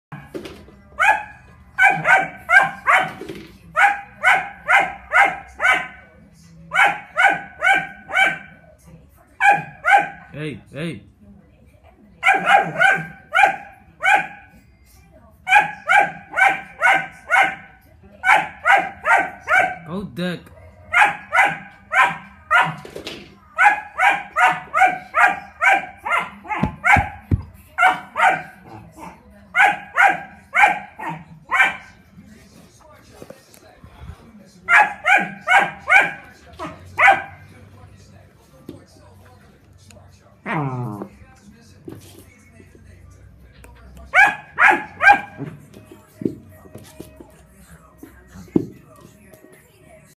11 week old gsd vs broom (i live in a apartment)
xD cute gsd got him when he was 4 weeks lol